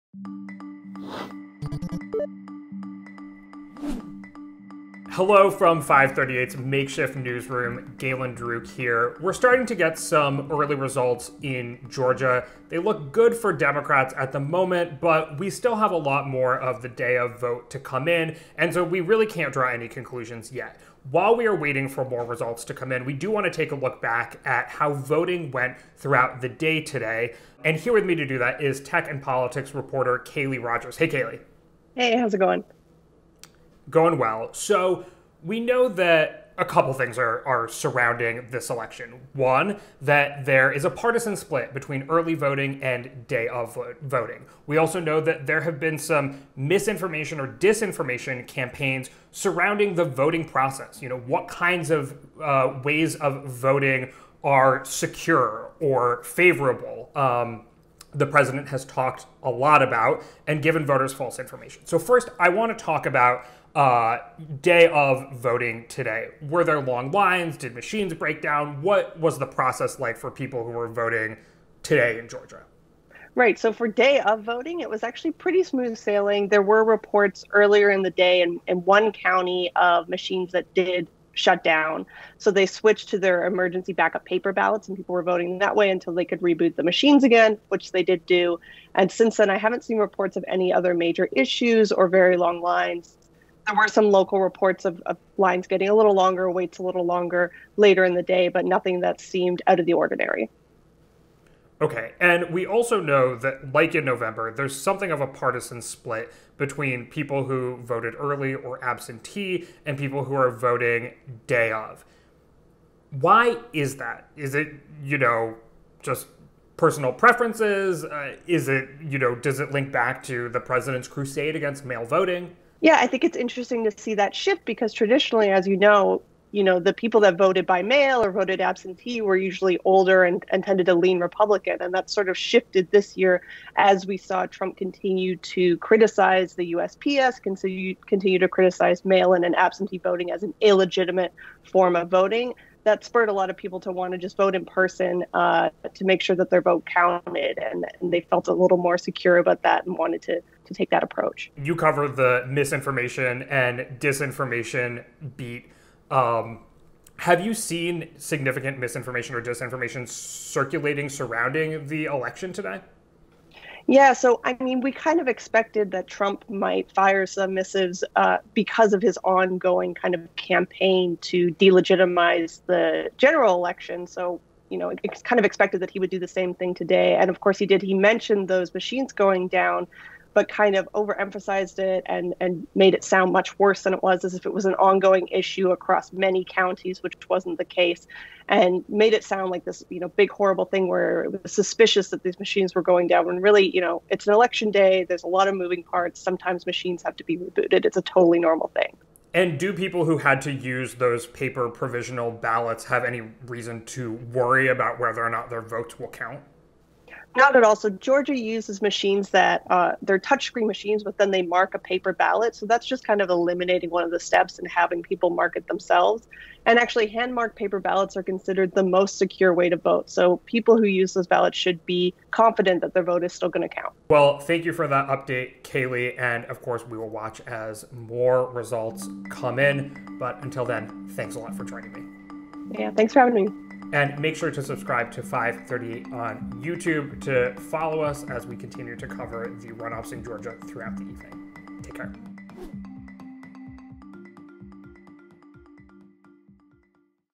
Despite False Claims, Voting Went Smoothly In Georgia l FiveThirtyEight
Galen chats with tech and politics reporter Kaleigh Rogers about the early vote in Georgia and whether broken voting machines should be a serious concern.